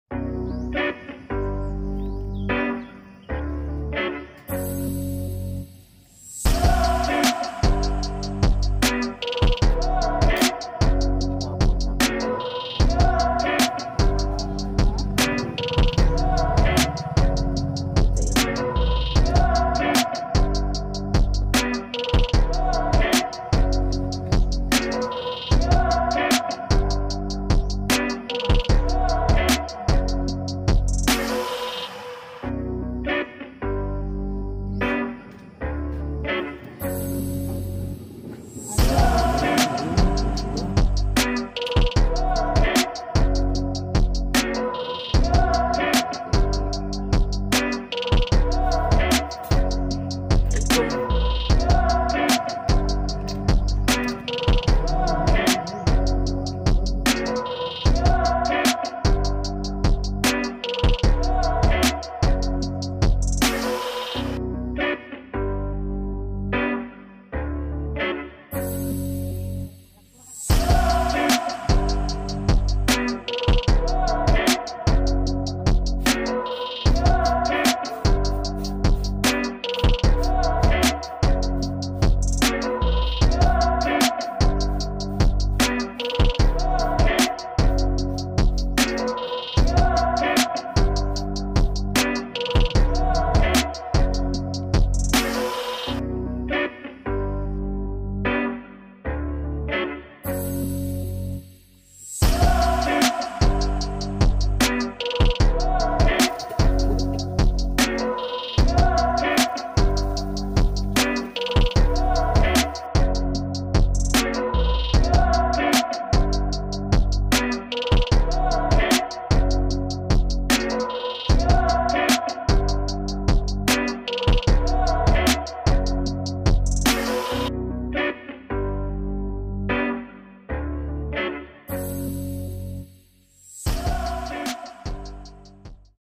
2 months change oil Honda click 125i v2
Gear oil at sentitic engine oil from honda